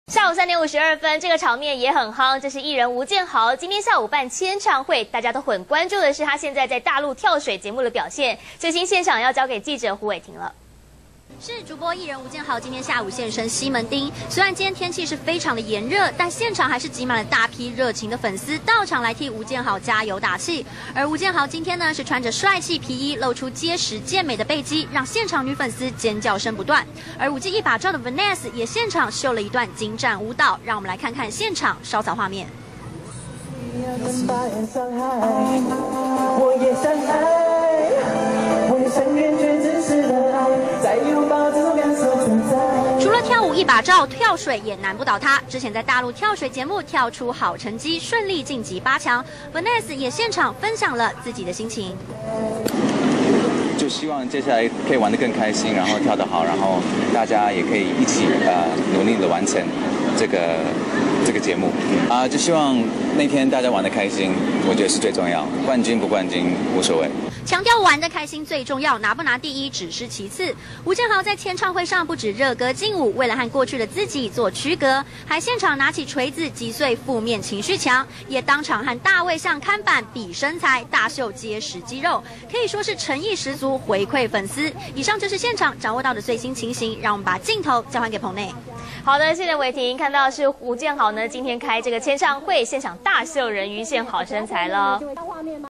中天新聞》吳建豪專輯簽唱會 大批粉絲到場支持
中天新聞》在大陸參加跳水節目表現優異備受關注的藝人吳建豪，下午舉辦專輯預購簽唱會。雖然今天天氣炎熱，現場還是擠滿大批粉絲到場支持。